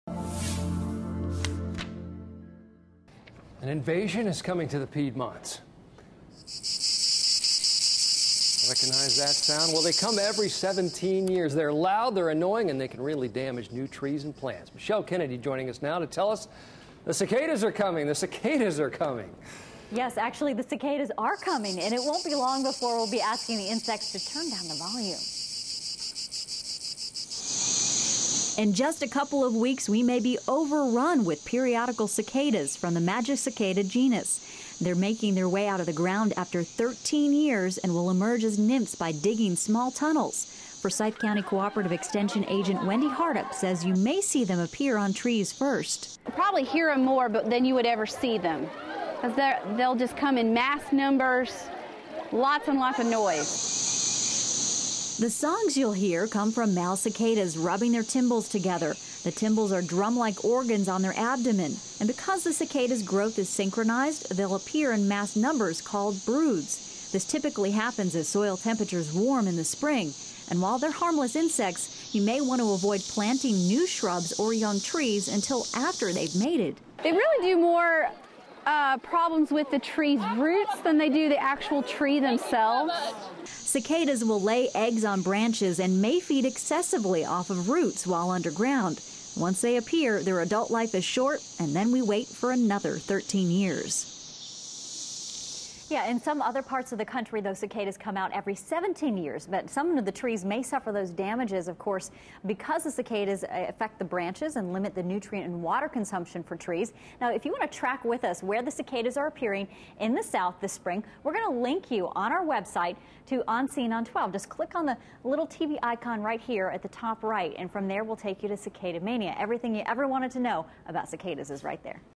The Cicadas Are Coming
They could appear soon as a swarm of insects on your windows and doors, and you may hear their cricket-like mating songs from down the street.